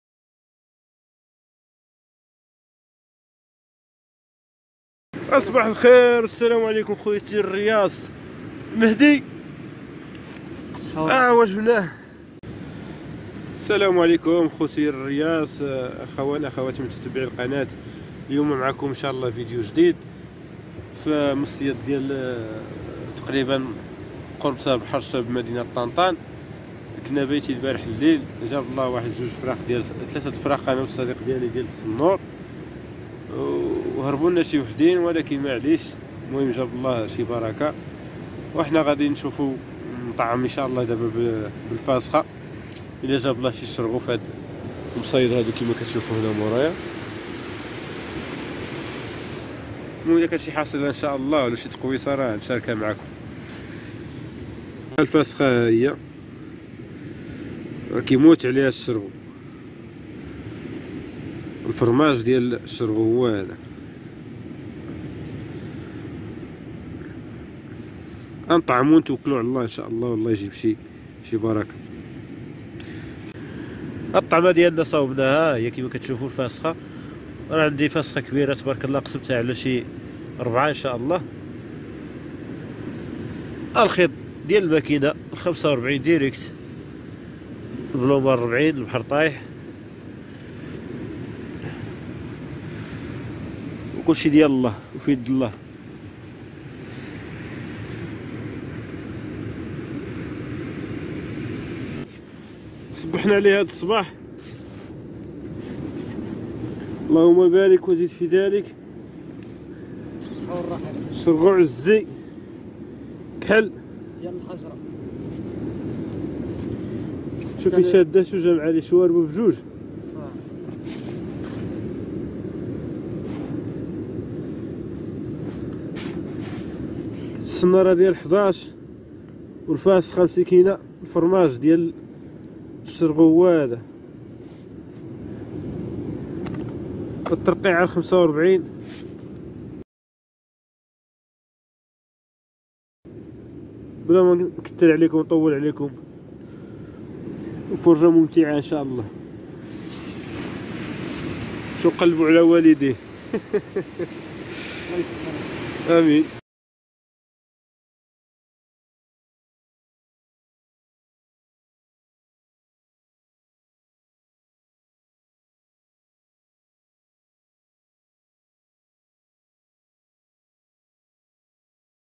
احسن طعم مع افضل مونطاج لصيد الشرغو بطريقة سهلة وفعالة
للإستمتاع والتعلم . شاهد ولن تندم